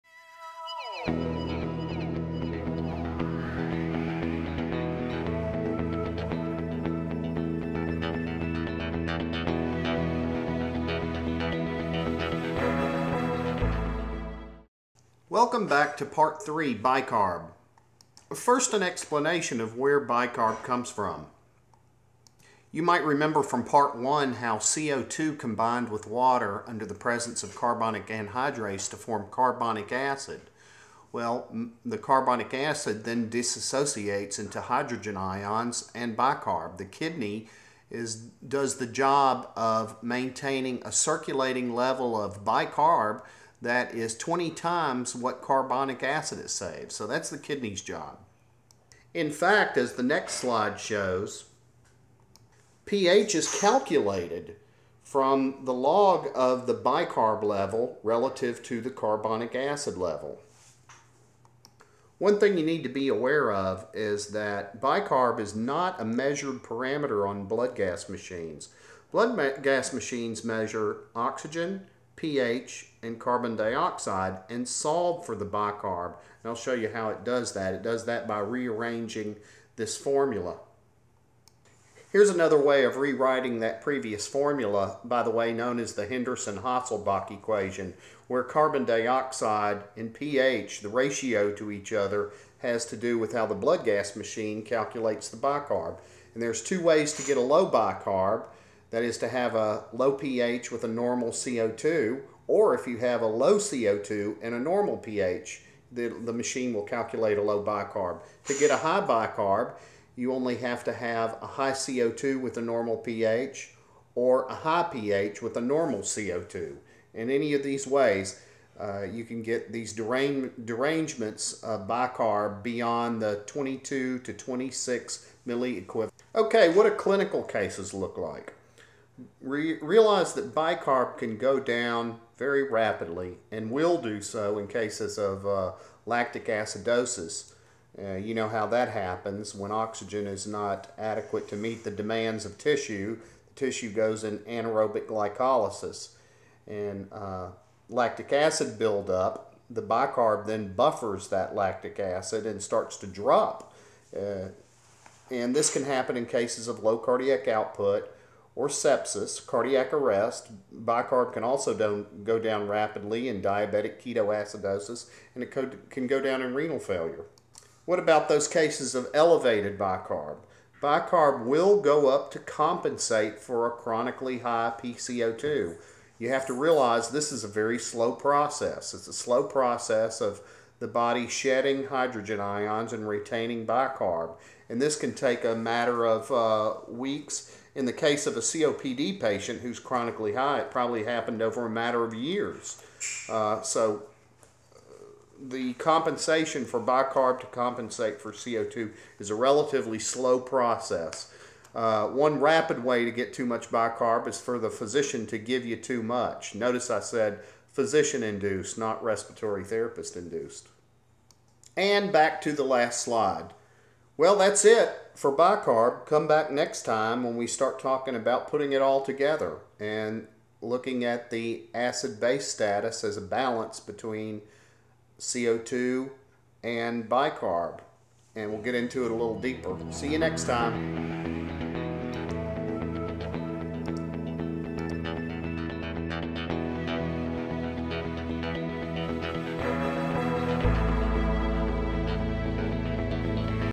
Clinical Application of Blood Gases, Part 3: Bicarb
Interpretation of Arterial Blood Gases is  critical for Respiratory Therapists and Nurses working in critical care...